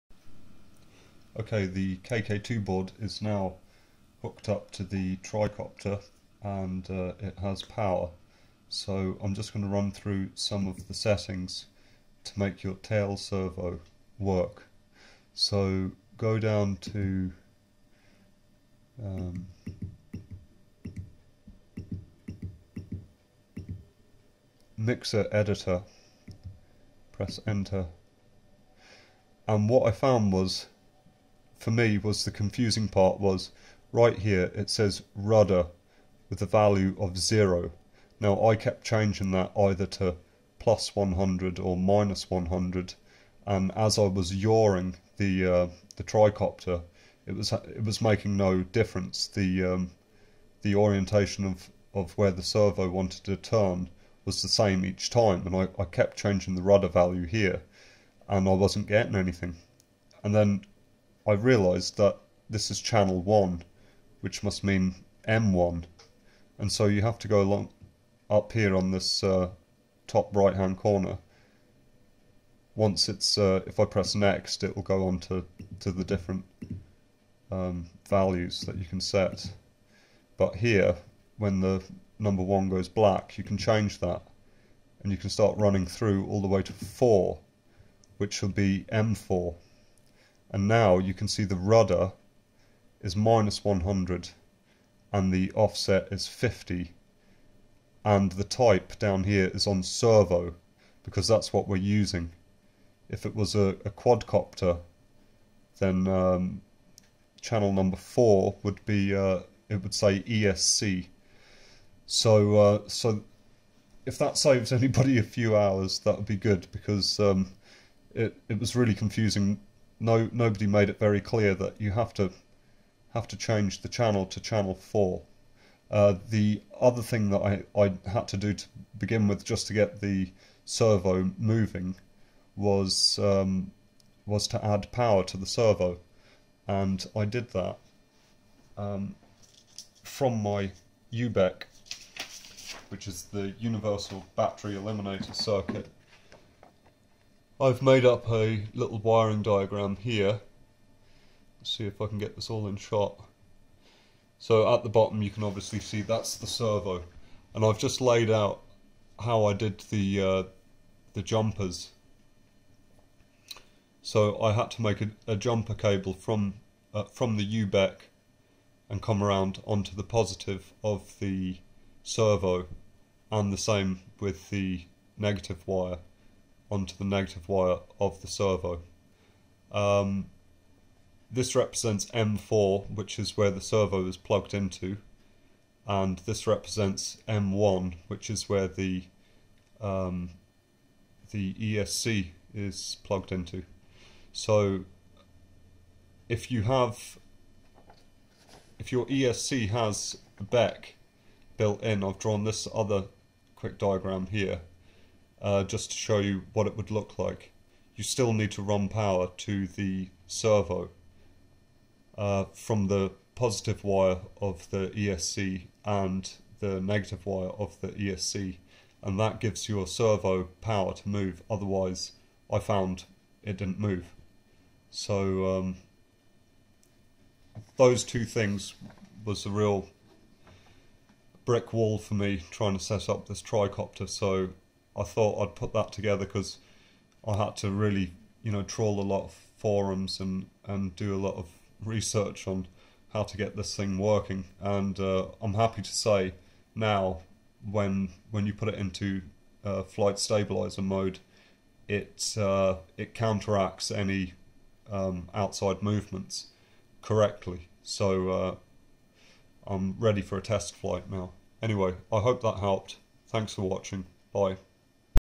Kk2.0 tricopter servo not responding reversing fix
If your tail servo is not responding here are two things you can try with the kk2 board.